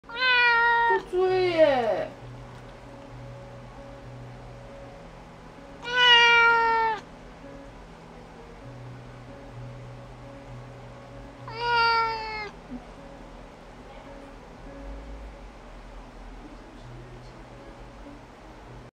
豆豆唉唉叫
後來收籠子時， 豆豆又慌張地四處找地方躲，後來躲進沙發後側的書櫃裡，叫得好可憐